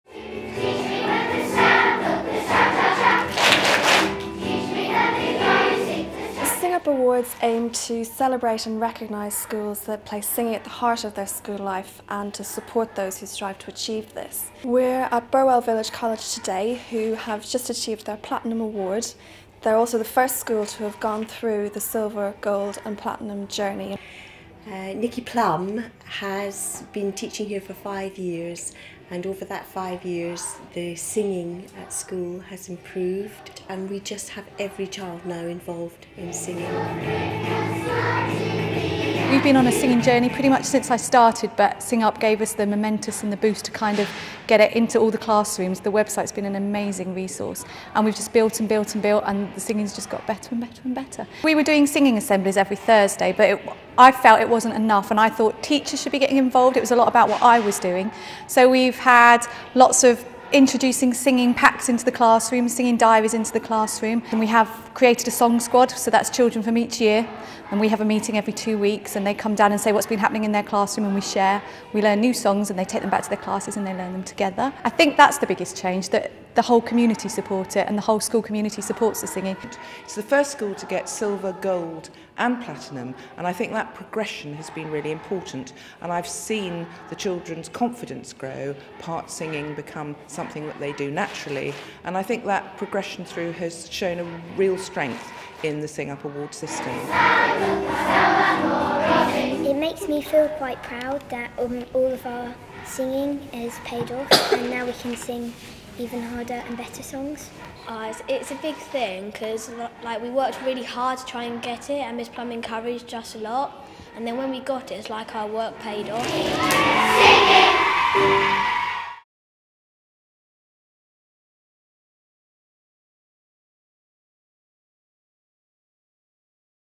Case Study - Burwell Village College Primary School, Sing Up Platinum School - Sing Up
Burwell Village College Primary School is the first Sing Up school to complete the Awards Journey achieving the Silver, Gold and Platinum Award. See what this achievement means to the school, and how they did it! To find out more and get started on your Awards Journey go